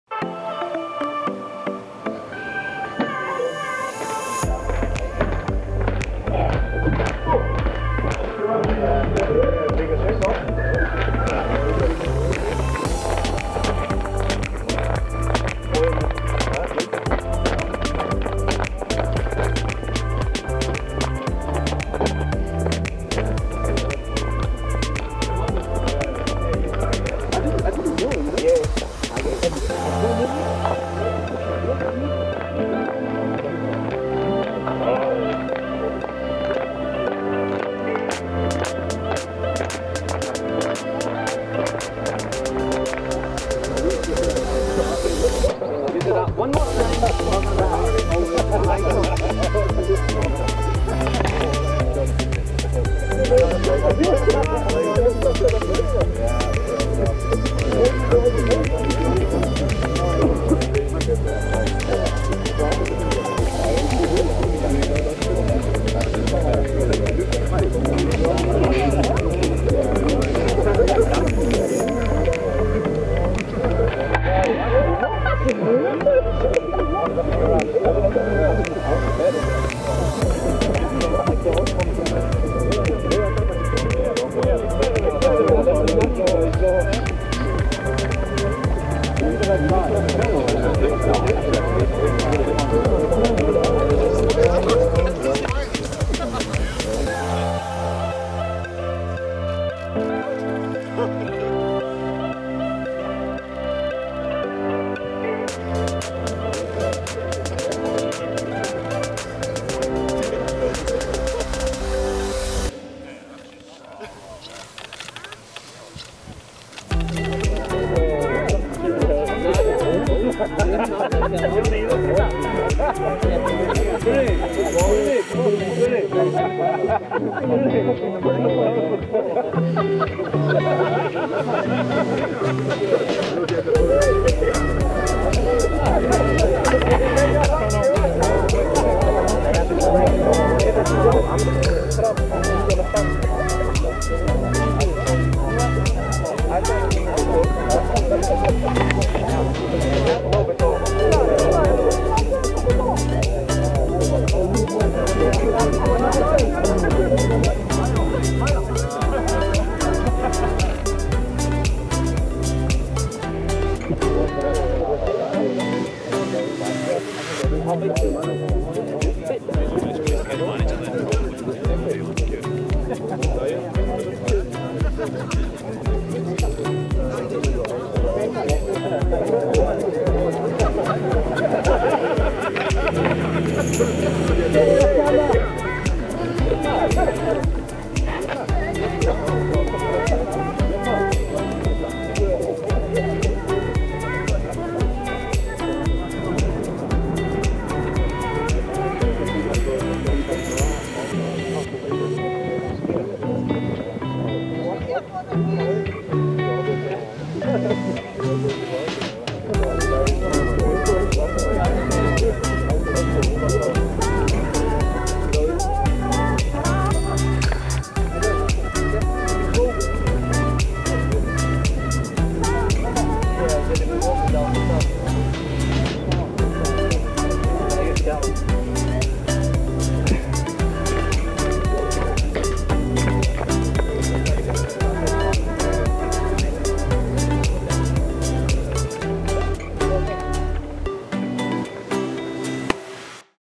Celtic 2019/20 Squad Photoshoot: Behind the Scenes
It was all smiles at Lennoxtown for the 2019/20 official team photoshoot. Go behind the scenes with the Bhoys.